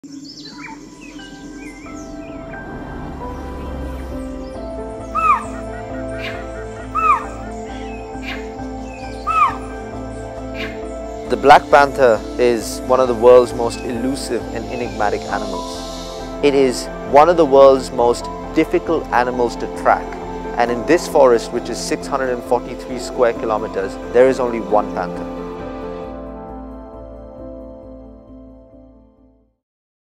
CNTx JLR: Kabini Teaser
Tracking the panther of Nagarhole through the forests of Kabini with wildlife photographer, Shaaz Jung